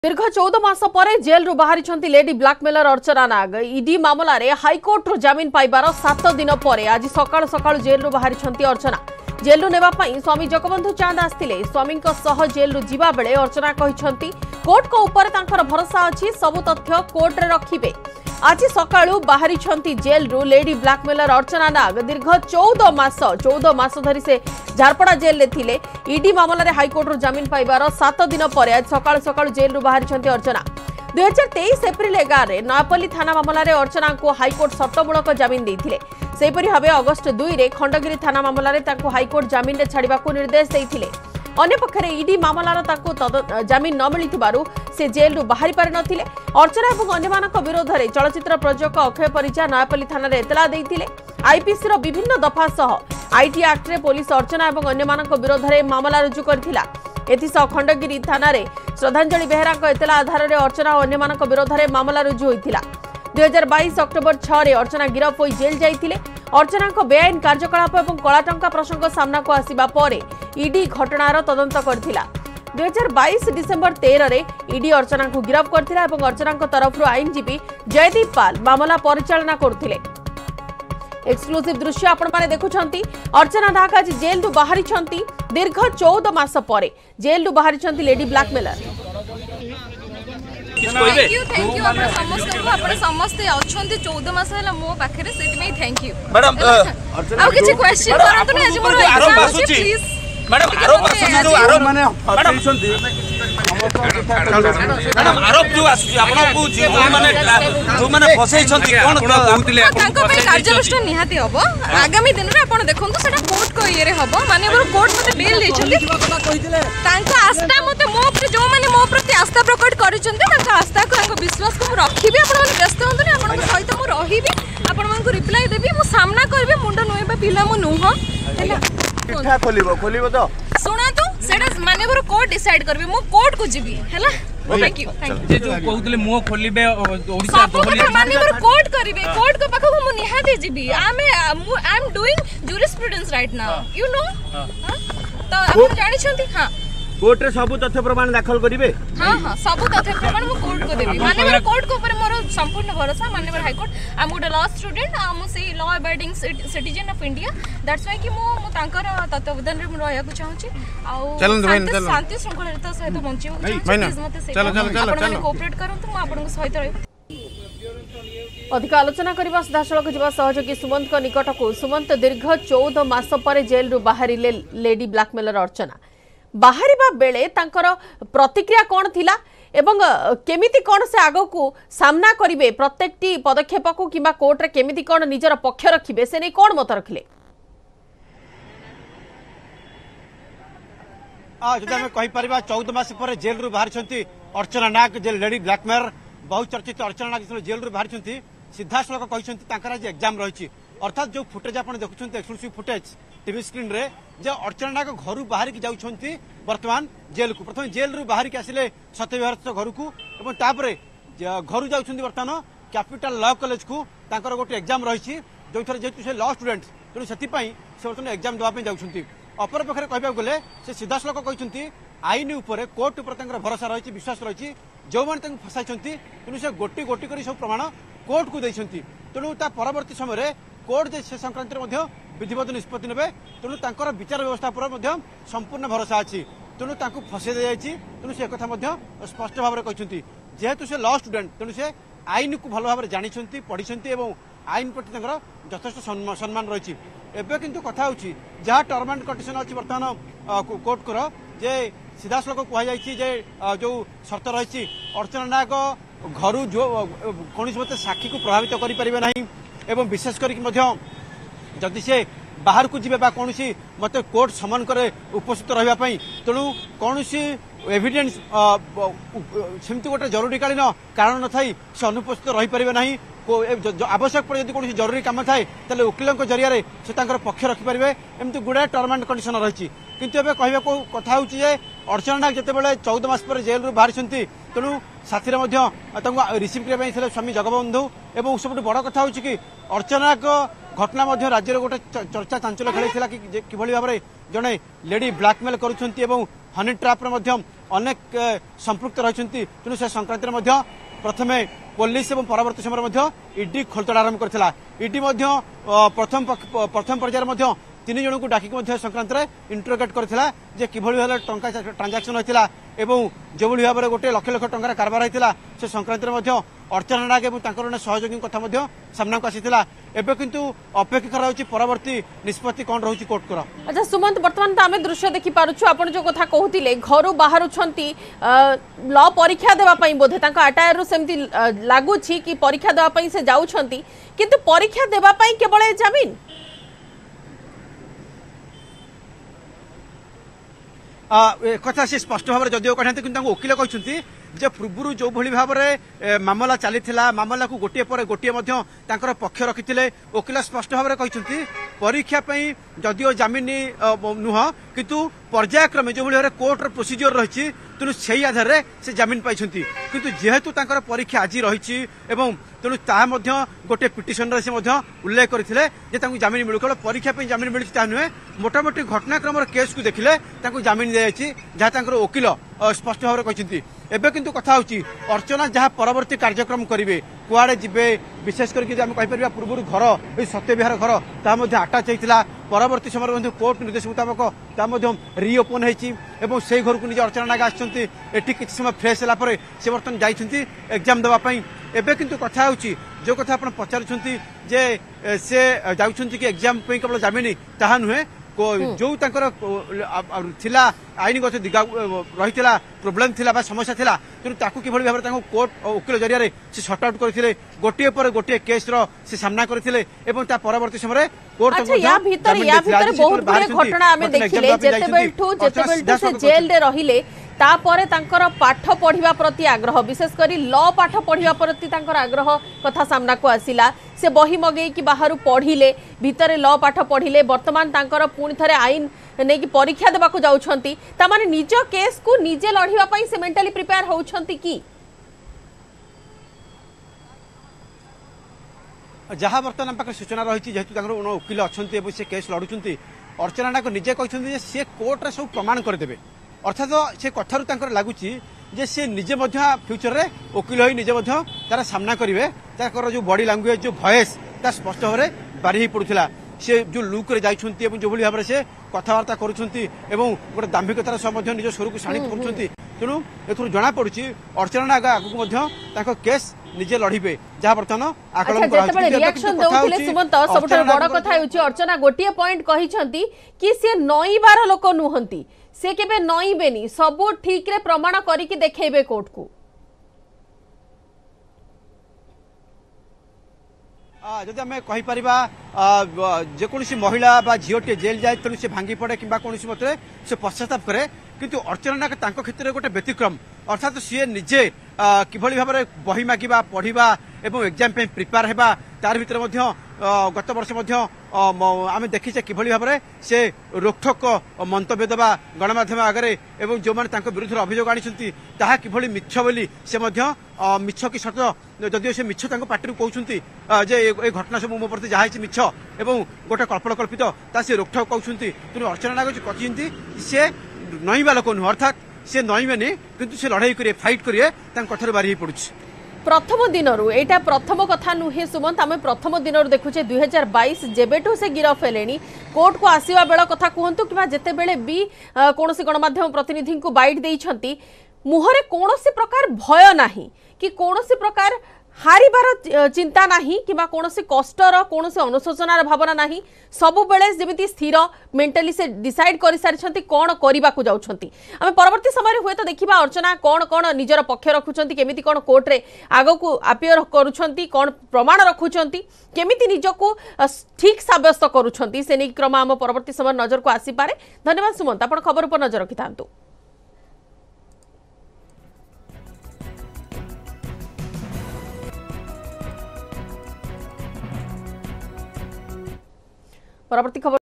Breaking News | ଜେଲ୍‌ରୁ ବାହାରିଲେ ନାଗ | Archana Nag walks out of jail On Bail | Odia News
Honey trap queen Archana Nag has finally walked out of Jharpada jail after 14 months of imprisonment. This comes seven days after the Orissa High Court granted bail to Nag on December 4. Archana Nag, along with her bailer who provided two lakh rupees as surety, has submitted their passports in court. Further, She has provided the address where she will reside, and any change in the residence without the court’s permission is not allowed.